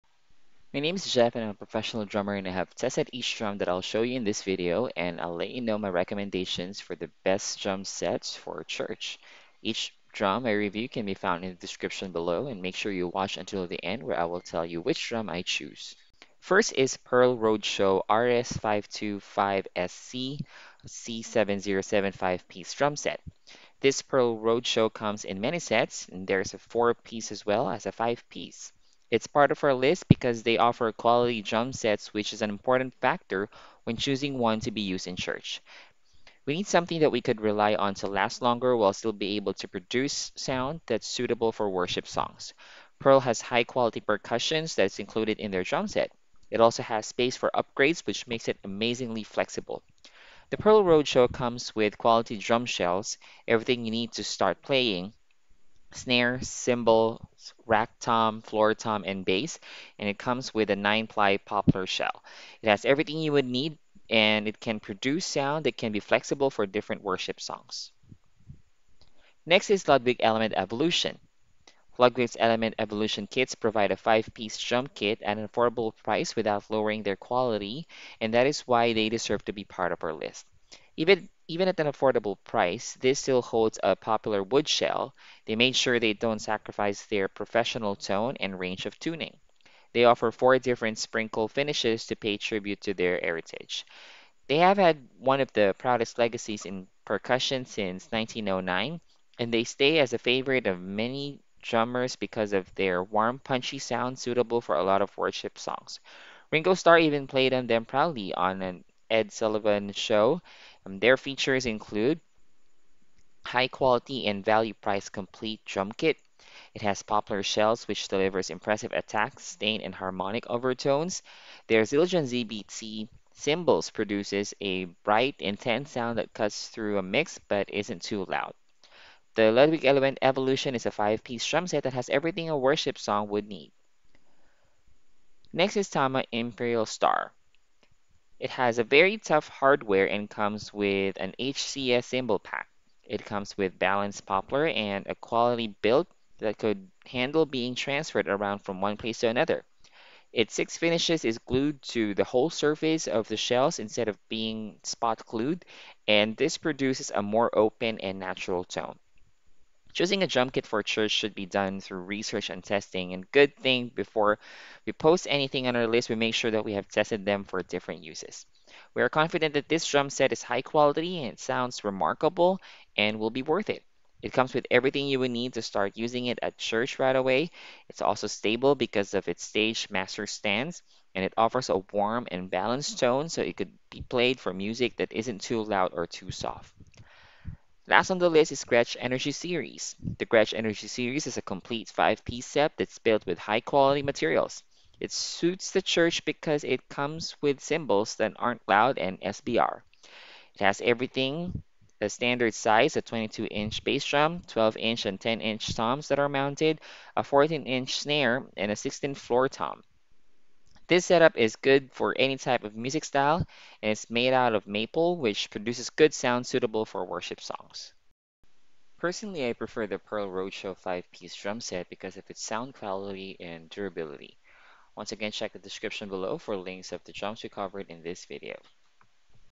Best Drum Sets for Church
Here are the top 4:
1. Pearl Roadshow RS525SC/C707 5-Piece Drum Set
2. Ludwig Element Evolution
3. TAMA Imperialstar
4. Gretsch Energy Series